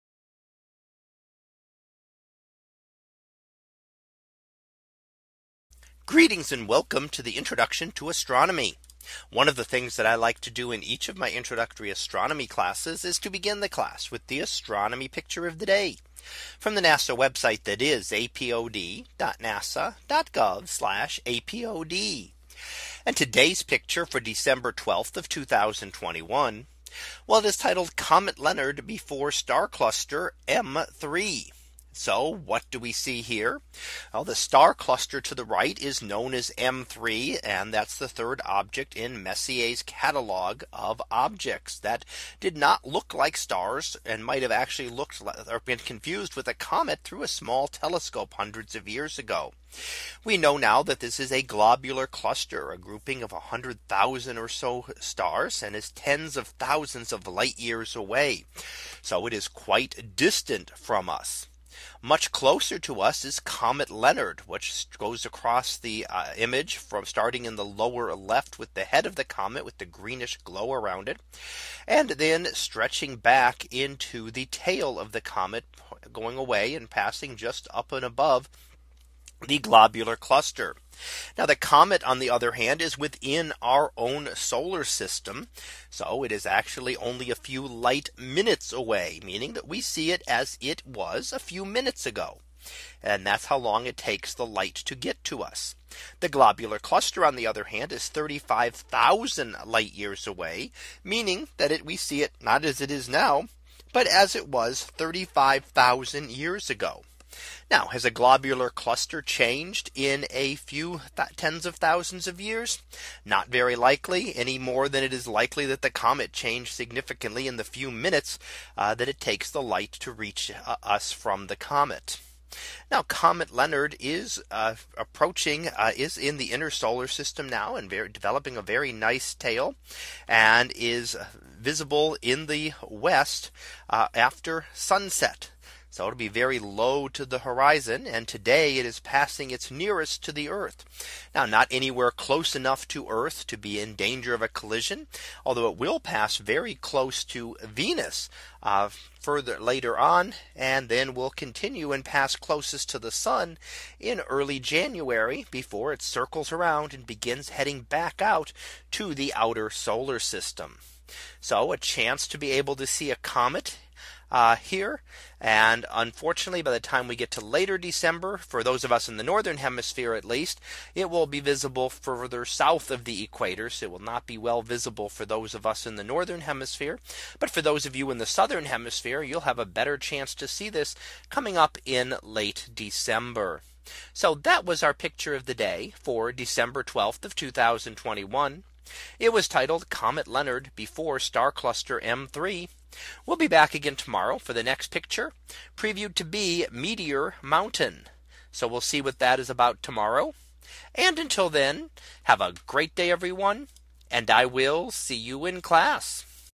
2021 December 12 - Comet Leonard Before Star Cluster M3
In today's image, we see Comet Leonard which makes its closest approach to Earth today. We also see a distant globular star cluster.